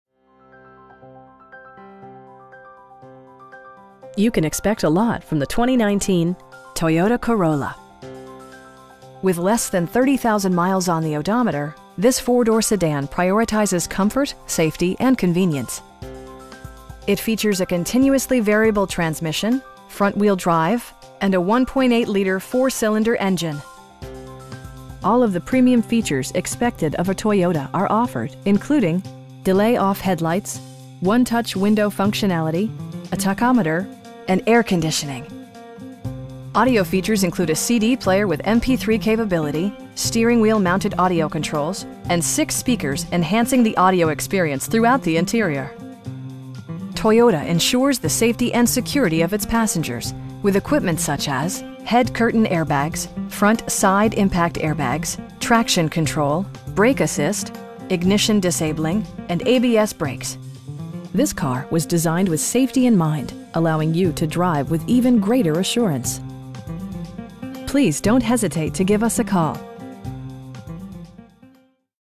2019 Toyota Corolla in Savannah, GA 31419
Vaden Nissan Savannah
10421 Abercorn St

You can expect a lot from the 2019 Toyota Corolla. With less than 30,000 miles on the odometer, this 4 door sedan prioritizes comfort, safety and convenience. It features a continuously variable transmission, front-wheel drive, and a 1.8 liter 4 cylinder engine. All of the premium features expected of a Toyota are offered, including: delay-off headlights, 1-touch window functionality, a tachometer, front bucket seats, power door mirrors, power windows, and air conditioning. Audio features include a CD player with MP3 capability, steering wheel mounted audio controls, and 6 speakers, enhancing the audio experience throughout the interior. Toyota ensures the safety and security of its passengers with equipment such as: head curtain airbags, front side impact airbags, traction control, brake assist, ignition disabling, and ABS brakes. This car was designed with safety in mind, allowing you to drive with even greater assurance. We pride ourselves on providing excellent customer service. Please don't hesitate to give us a call.